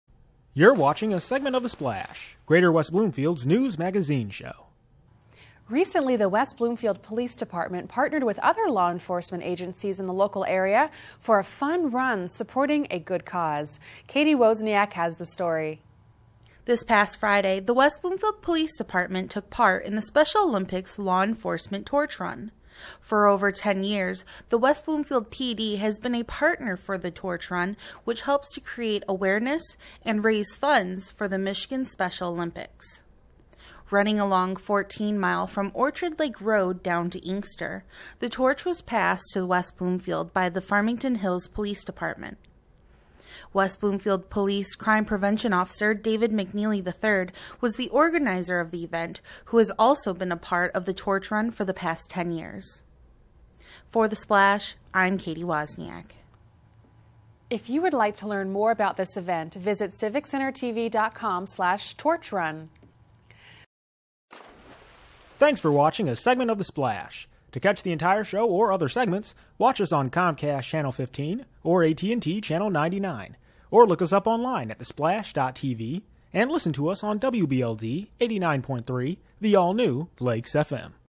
Splash Feature: Special Olympics Torch Run
Splash Reporter Cady Wozniak covers the West Bloomfield Police Department’s partnership with other neighboring law enforcement agencies in a fun run to support the Special Olympics.